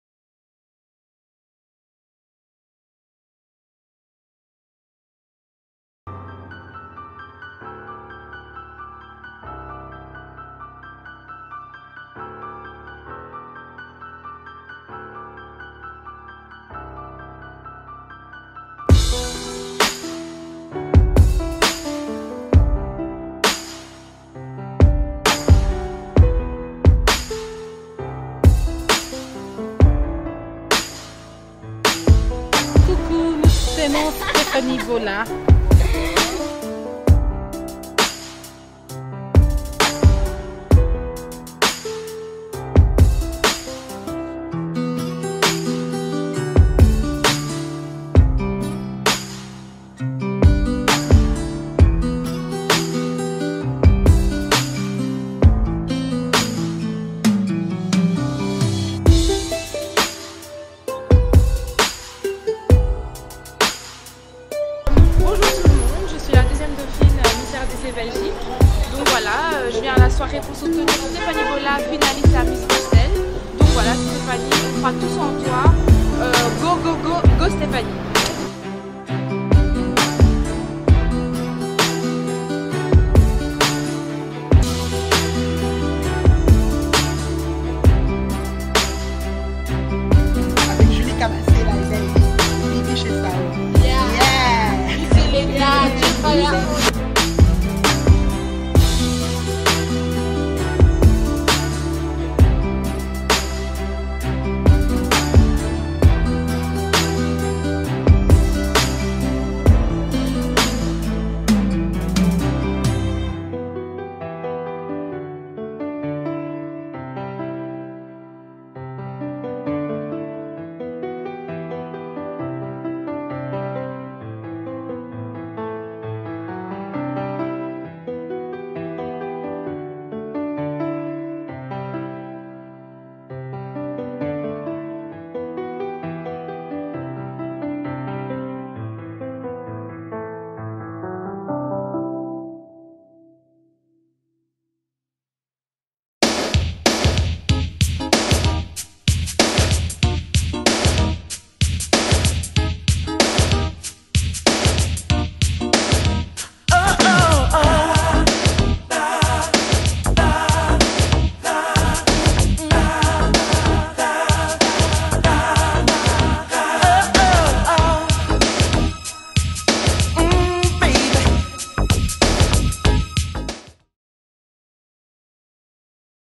Stephanie Bola & GFAIA
GFAIA soutient Stephanie Bola au cours d'une soirée "Old School "